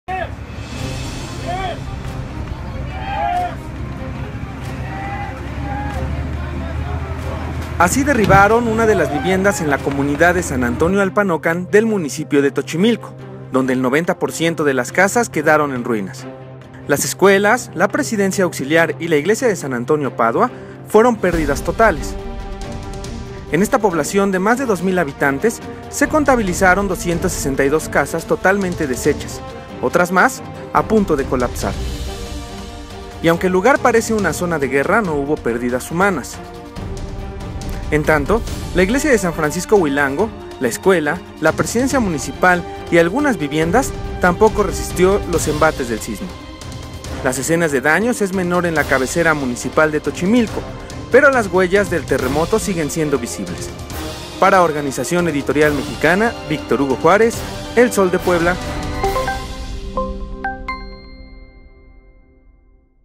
En San Antonio Alpanocan el 90% de las viviendas se dañaron
El sismo les quitó su patrimonio en segundos